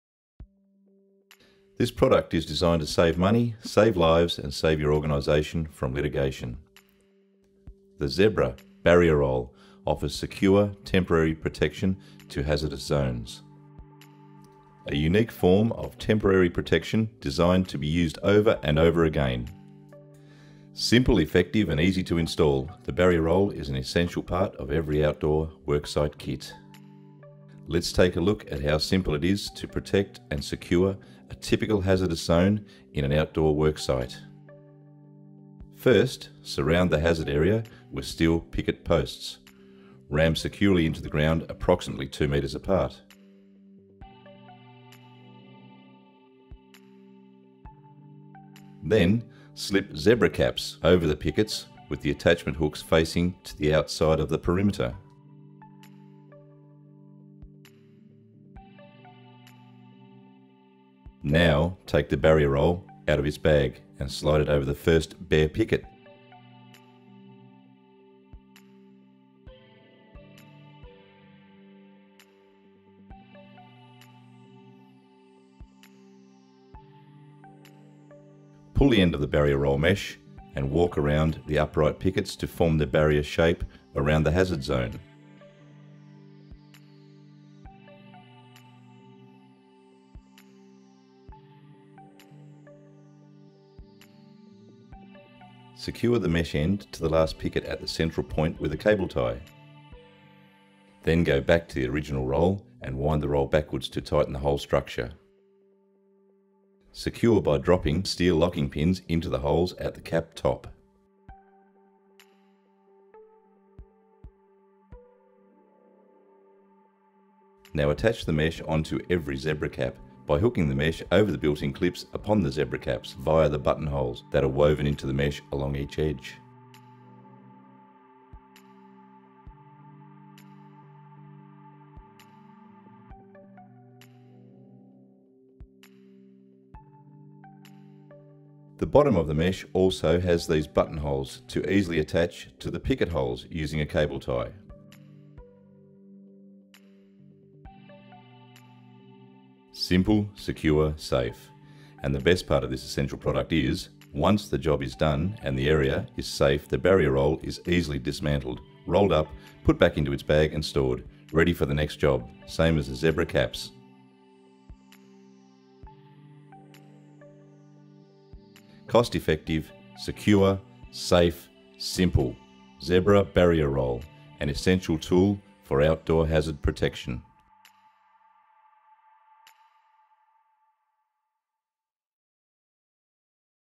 Barrier Roll Zone Demarcation Barrier Mesh
Benefits:

• No more loose, unsightly mesh
• Can be re-used! Just roll it up, put into a special bag and take to the next site
• More secure - blocks off the hazard
• Keeps the dust inside Makes the hazard more visible, which reduces liability risks